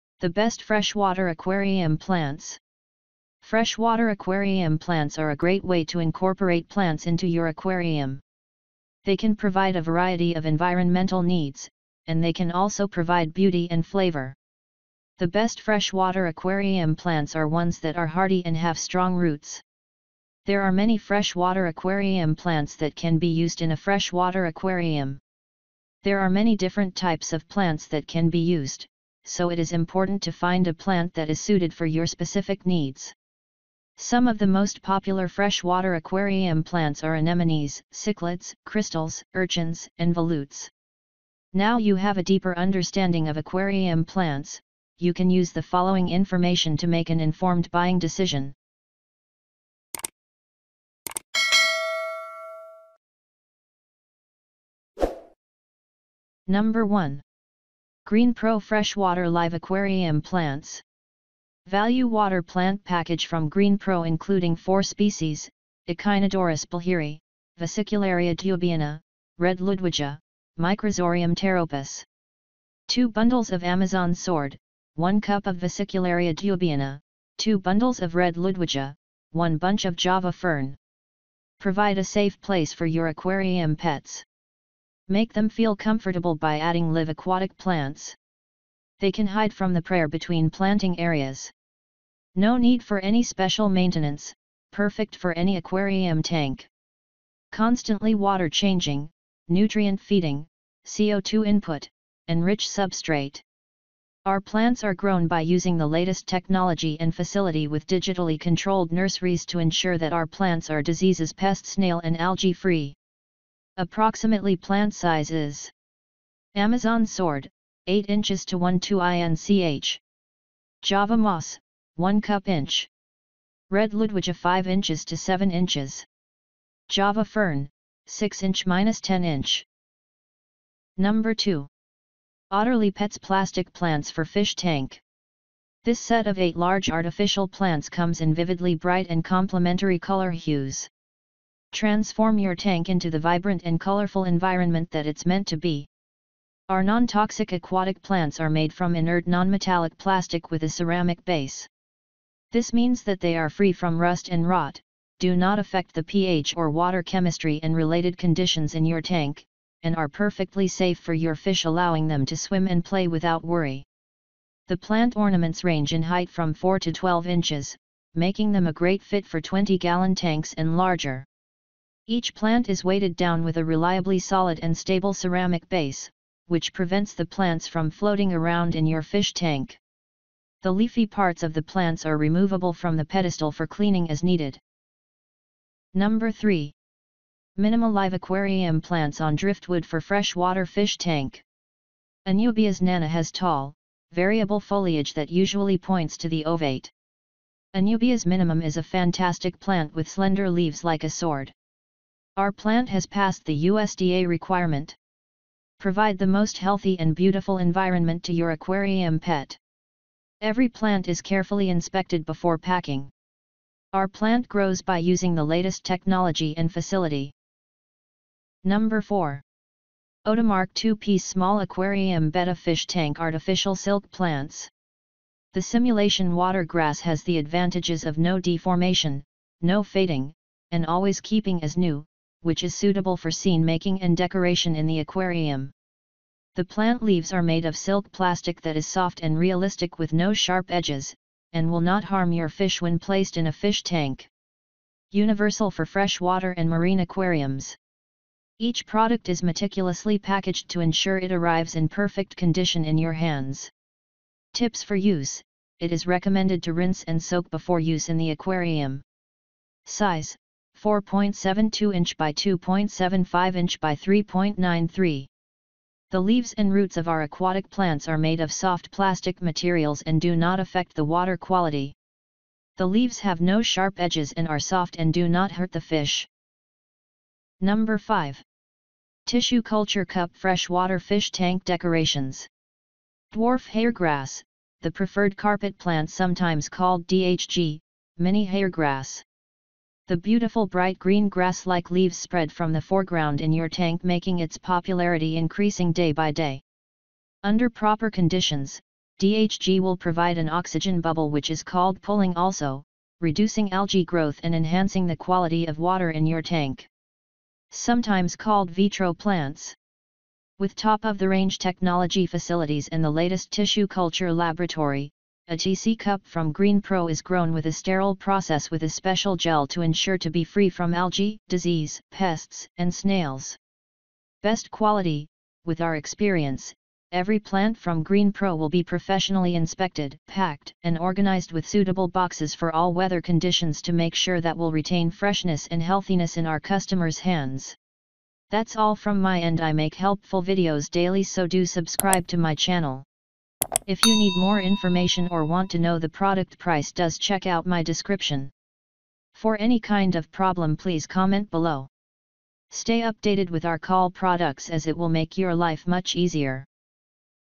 ✅ Top 5 Best Freshwater Aquarium Plants Reviews
👉1. Greenpro Freshwater Live Aquarium Plants

👉2. Otterly Pets Plastic Plants for Fish Tank

👉3. Driftwood for Freshwater Fish Tank

👉4. Betta Fish Tank Artificial Silk Plants

 00:00 The Best Freshwater Aquarium Plants.

Freshwater aquarium plants are a great way to incorporate plants into your aquarium. They can provide a variety of environmental needs, and they can also provide beauty and flavor. The best freshwater aquarium plants are ones that are hardy and have strong roots.

There are many freshwater aquarium plants that can be used in a freshwater aquarium. There are many different types of plants that can be used, so it is important to find a plant that is suited for your specific needs. Some of the most popular freshwater aquarium plants are anemones, cichlids, crystals, urchins, and volutes.

Now you have a deeper understanding of aquarium plants, you can use the following information to make an informed buying decision.

  00:56 Number 1. Greenpro Freshwater Live Aquarium Plants.

Value water plant package from Greenpro including 4 species, Echinodorus Bleheri, Vesicularia Dubyana, Red Ludwigia, Microsorium Pteropus.

2 Bundles of Amazon Sword, 1 cup of Vesicularia Dubyana, 2 Bundles of Red Ludwigia, 1 Bunch of Java Fern.

Provide a safe place for your aquarium pets. Make them feel comfortable by adding live Aquatic plants. They can hide from the prayer between planting areas.

Our plants are grown by using the latest technology and facility with digitally controlled nurseries to ensure that our plants are diseases pest snail and algae-free.

  02:11 Number 2. Otterly Pets Plastic Plants for Fish Tank.

This set of 8 large artificial plants comes in vividly bright and complimentary color hues. Transform your tank into the vibrant and colorful environment that it's meant to be.

Our non-toxic aquatic plants are made from inert non-metallic plastic with a ceramic base. This means that they are free from rust and rot, do not affect the PH or water chemistry and related conditions in your tank, and are perfectly safe for your fish allowing them to swim and play without worry.

The plant ornaments range in height from 4 to 12 inches, making them a great fit for 20-gallon tanks and larger.

Each plant is weighted down with a reliably solid and stable ceramic base, which prevents the plants from floating around in your fish tank. The leafy parts of the plants are removable from the pedestal for cleaning as needed.

 03:05 Number 3.  Minima Live Aquarium Plants on Driftwood for Freshwater Fish Tank.

Anubias nana has tall, variable foliage that usually points to the ovate. Anubias minimum is a fantastic plant with slender leaves like a sword.
Our plant has passed the USDA requirement.

Provide the most healthy and beautiful environment to your aquarium pet.
Every plant is carefully inspected before packing.
Our plant grows by using the latest technology and facility.

 03:39 Number 4.  Otimark 2 Piece Small Aquarium Betta Fish Tank Artificial Silk Plants.

The simulation water grass has the advantages of no deformation, no fading, and always keeping as new, which is suitable for scene making and decoration in the aquarium.

The plant leaves are made of silk plastic that is soft and realistic with no sharp edges, and will not harm your fish when placed in a fish tank. Universal for freshwater and marine aquariums!

  04:37 Number 5.   Tissue Culture Cup Freshwater Fish Tank Decorations.

Dwarf Hairgrass, the preferred carpet plant sometimes called DHG, Mini Hairgrass. The beautiful bright green grass-like leaves spread from the foreground in your tank making its popularity increase day by day.

You Can Buy it Right Now. We Made This List Based On Our Personal Opinion and hours of research & we have listed them based on the type of features and Price. We have included options for every type of user so whether you are looking for the best budget.